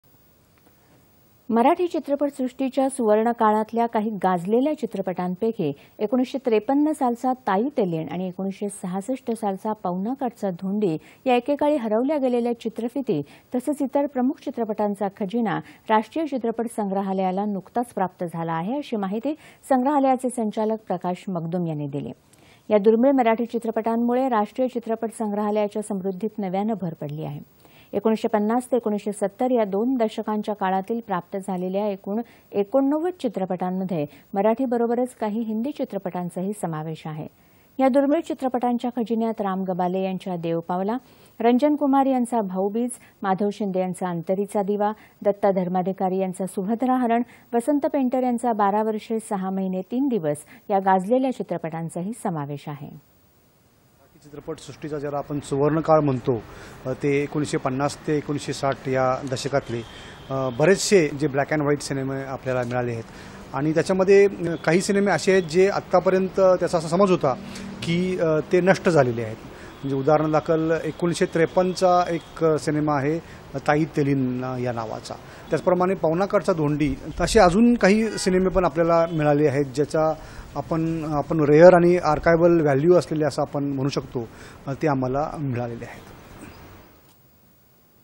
'चित्रपटांचा खजिना राष्ट्रीय चित्रपट संग्रहालयाला नुकताच प्राप्त झाला आहे
मराठी चित्रपटसृष्टीच्या सुवर्णकाळातल्या काही गाजलेल्या चित्रपटांपैकी  १९५३ सालचा 'ताई तेलीण'  आणि १९६६ सालचा
   'पवनाकाठचा धोंडी' या एकेकाळी 'हरवल्या' गेलेल्या चित्रफिती तसंच इतर प्रमुख 'चित्रपटांचा खजिना राष्ट्रीय चित्रपट संग्रहालयाला नुकताच प्राप्त झाला आहे, अशी माहिती संग्रहालयाचे संचालक प्रकाश मगदुम यांनी दिली.
    या दुर्मीळ मराठी चित्रपटांमुळे राष्ट्रीय चित्रपट संग्रहालयाच्या समृद्धीत नव्याने भर पडली आहे. १९५० ते १९७० या दोन दशकांच्या काळातील प्राप्त झालेल्या एकूण ८९ चित्रपटांमध्ये मराठीबरोबरच काही हिंदी चित्रपटांचाही समावेश आहे. या दुर्मीळ चित्रपटांच्या खजिन्यात राम गबाले यांच्या 'देव पावला' , रंजन कुमार यांचा 'भाऊबीज', माधव शिंदे यांचा 'अंतरीचा दिवा', दत्ता धर्माधिकारी यांचा 'सुभद्राहरण", वसंत पेंटर यांचा 'बारा वर्षे, सहा महिने तीन दिवस या गाजलेल्या चित्रपटांचाही समावेश आहे.
सिंक --- प्रकाश मगदुम,संचालक, राष्ट्रीय फिल्म संग्रहालय, पुणे